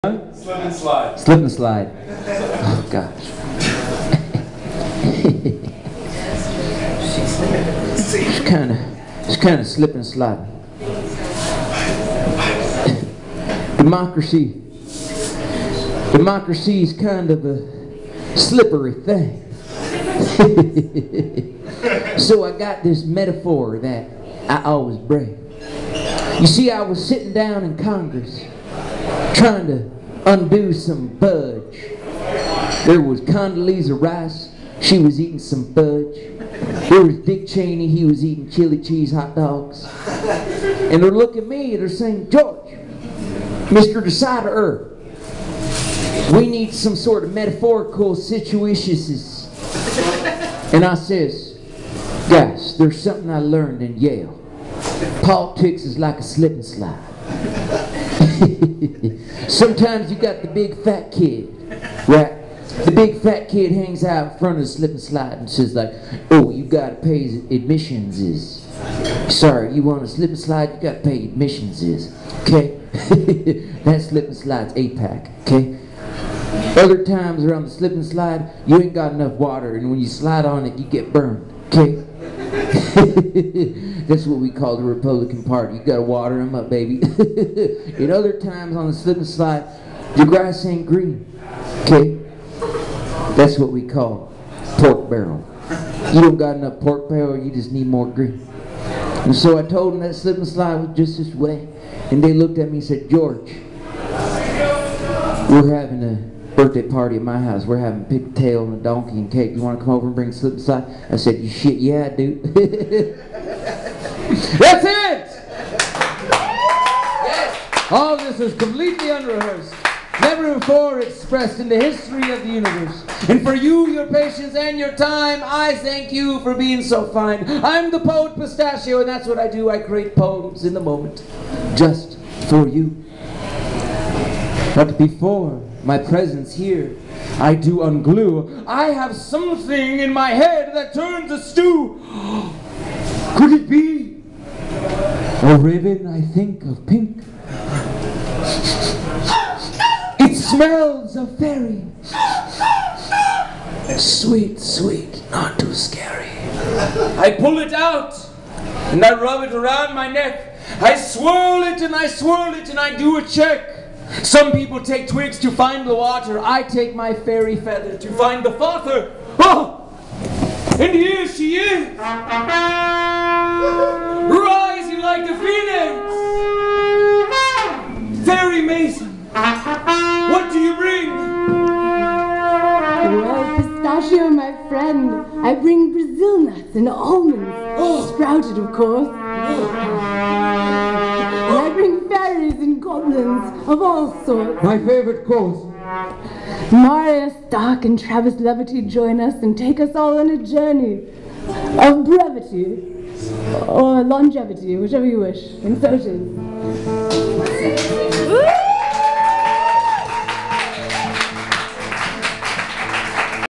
Uncle Sedgwick's Curious Spectacle, Poet Pistachio, Stark Levity @ Stonehouse 2013
"Uncle Sedgwick's Curious Spectacle?"
At a run time of three hours, the multiple act performance takes the audience on a whimsical journey complete with music, poetry, and theatre. Dramatic and comedic, sewn with threads of levity and the gravitas of our deepest emotional seas, the Spectacle encompasses the full possibility of what live performance can be, and attempts go beyond it. Partly improvised, partly meticulously rehearsed, each Spectacle is as unique as the acts it is comprised of: PS (a 7 Piece Americana Fantastica Folk Band), The Poet Pistachio (Wisdom and Hilarity Through Improvised Lyrical Poems), and Stark Levity (Dark Fairy Canticles from an Archaic Bard Duo) are excited to feed the people with a most certainly brave form of entertainment expression!

The evening is not to be missed! A splendid time guaranteed for all!

The Stone House
Nevada City, CA
Thursday November 7th, 2013

Nikko Wu

Stonehouse Old Brewery
107 Sacramento St. Nevada City, CA 95959 USA